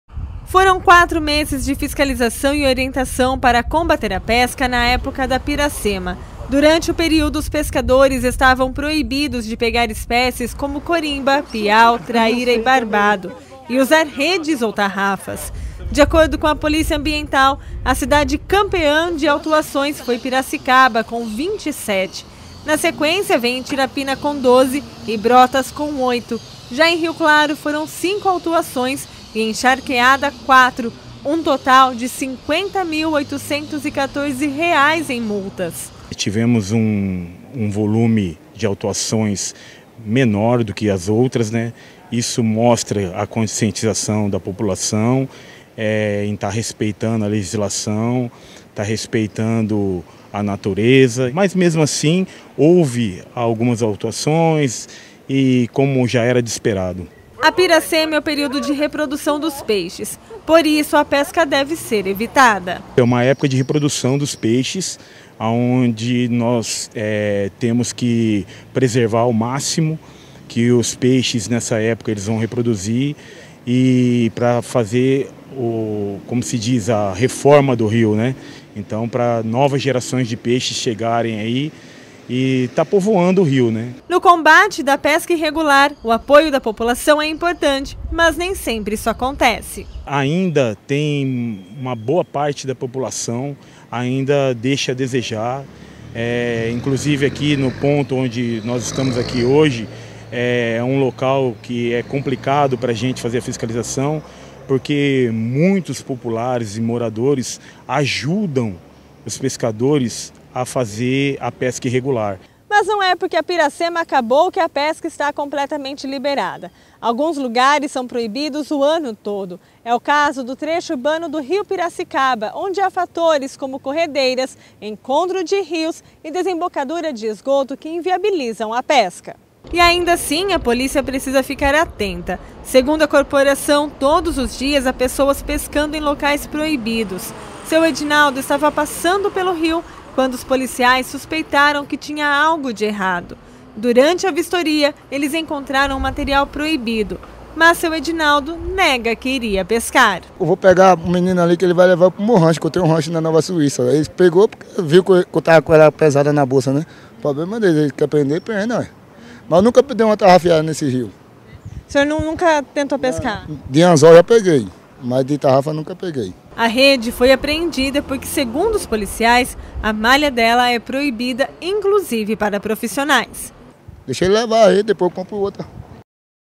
Polícia Ambiental aplica R$ 50 mil em multa durante a Piracema
Período é importante para a reprodução dos peixes. Neste período, a cidade que mais teve autuações foi Piracicaba.